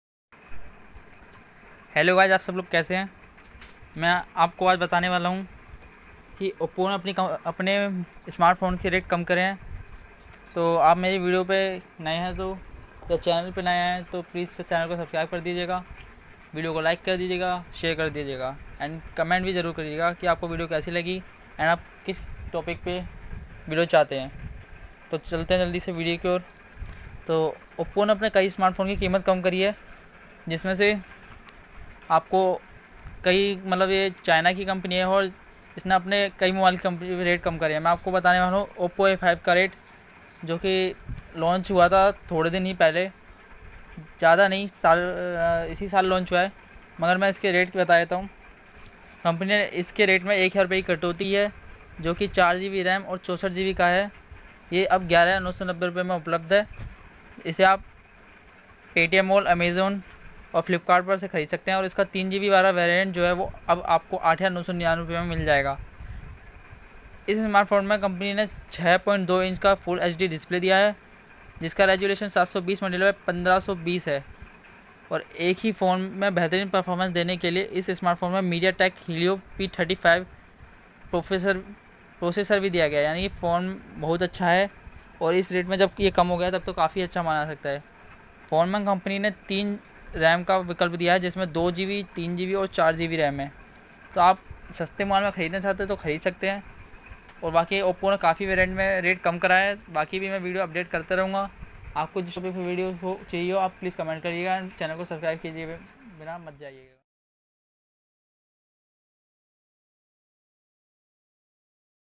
Oppo A5s slashed price| big mobile in Budget | month end sale
Oppo A5s slashed price
Oppo A5s price drop sale
Month end sale July 2019
Big mobile in Budget

Is video mai oppo ki month end sale ko bataya Gaya hai
Is video Mai oppo A5s ki Kam kimat ko bataya Gaya hai
Aap mobile ko online kisi bhi site se Khalid sakte hai
Flipkart, Amazon, Snapdead, etc.

Hello guyz 🙏
 Thanks,
 App Tech KK Gupta G